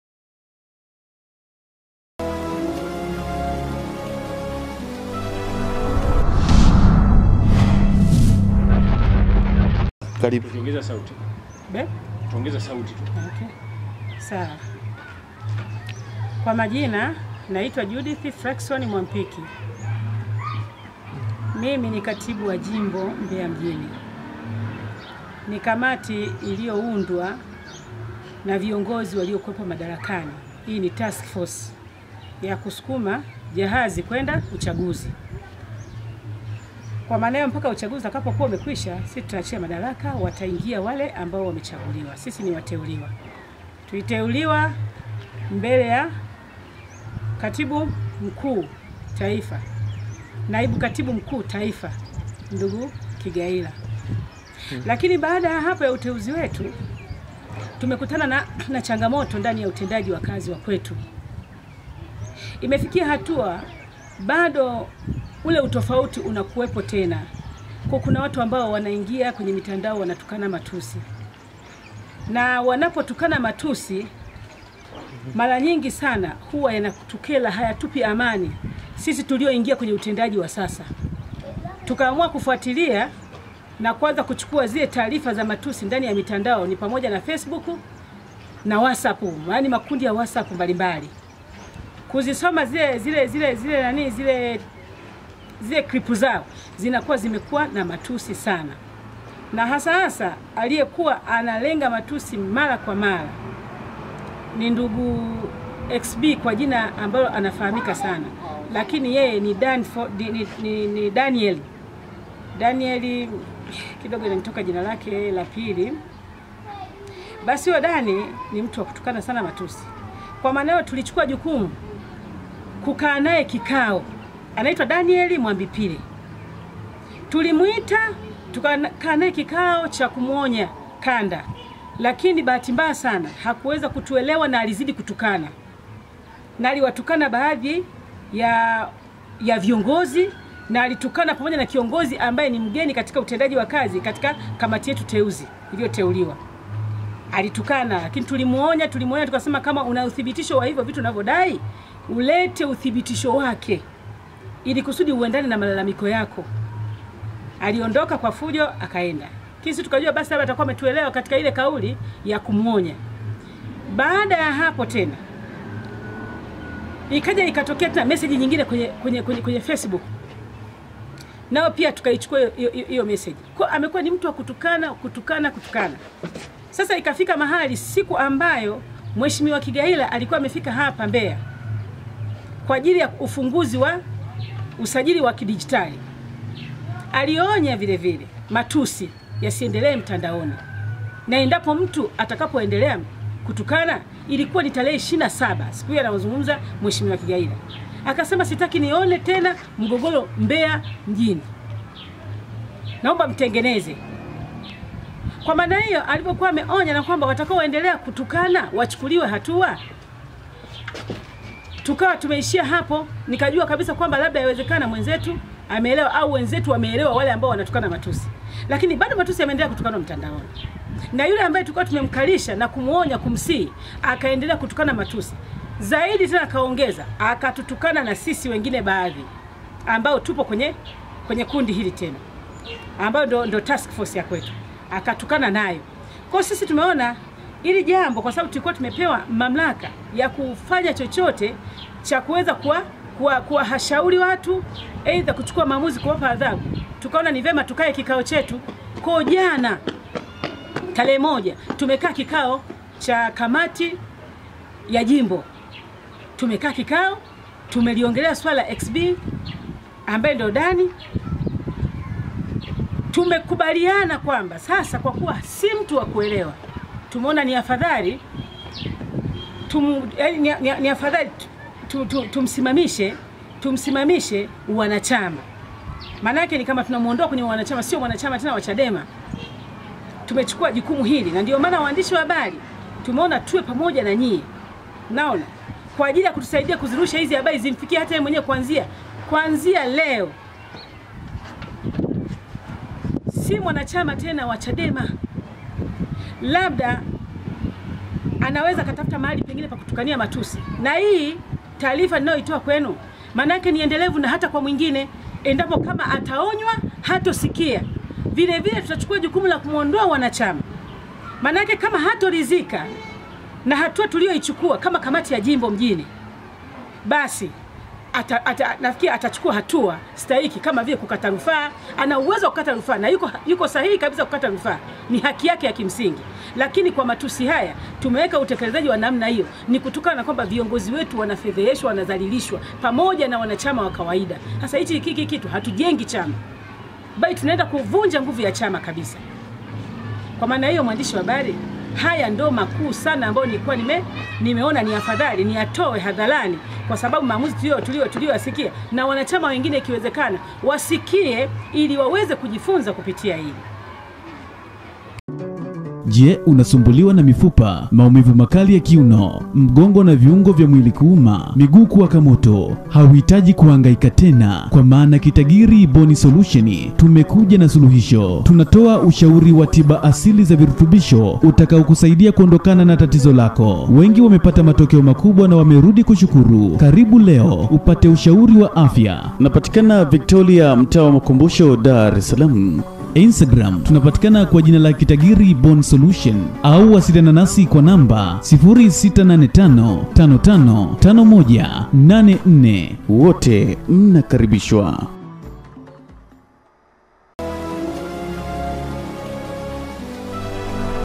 CHADEMA Mbeya ,Yawaonya Wanachama Wenye Matusi Mitandaoni,Mmoja avuliwa Uanachama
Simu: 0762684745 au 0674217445
Karibu sana